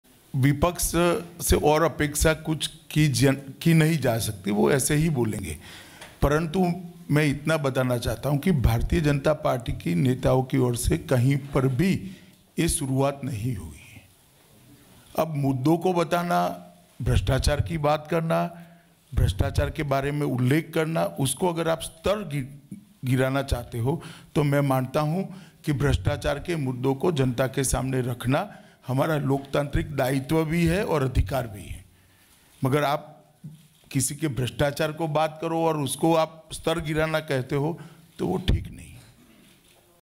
Amit Shah: "विपक्ष से कोई अपेक्षा नहीं, मुद्दों को बताना, भ्रष्टाचार की बात करना स्तर गिराना नहीं"
विपक्ष के बीजेपी पर स्तर गिराने के आरोप पर अमित शाह का विपक्ष पर हमला | सातवें चरण के चुनाव प्रचार के अंतिम दिन प्रधानमंत्री नरेंद्र मोदी और भाजपा अध्यक्ष अमित शाह प्रेस कांफ्रेंस कर रहे हैं। पांच साल में ये पहली बार है जब पीएम मोदी किसी प्रेस कांफ्रेंस में मौजूद हैं। इस दौरान अमित शाह ने मोदी सरकार की उपलब्धियों को सामने रखा।

For all the latest news and development across India, watch News18 India LIVE TV.

देखिये 2019 के लोक सभा चुनावों पर हर बड़ी अपडेट न्यूज़ 18 इंडिया के साथ.